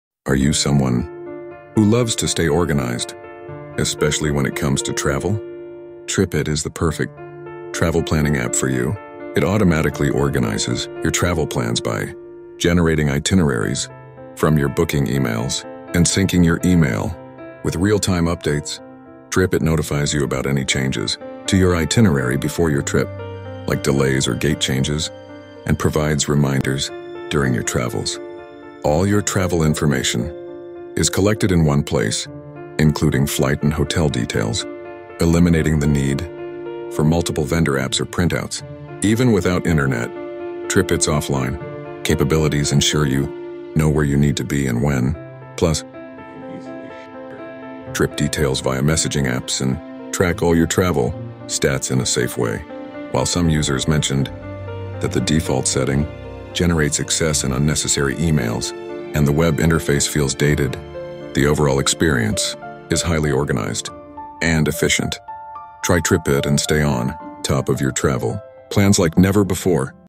How to use TRIPIT app before traveling
TripIt is a travel planning app that some say is great for people who like to be organized. It can help you:

Organize your travel plans

Get real-time updates

TripIt notifies you about changes to your itinerary, like delays or gate changes, before your trip. It also provides reminders about flight and check-in details during your travels.

Have all your travel information in one place

TripIt collects all your travel information, including flight and hotel information, so you don't have to use multiple vendor apps or printouts.

Use offline capabilities

TripIt can help you figure out where you're supposed to be and when, even if you don't have internet.

Here are some more insights from user and expert reviews:

Pros

Easy to share trip details via messaging apps

Tracks all of your travel stats

Safe

Cons

Generates excess and unnecessary email by default

Dated web interface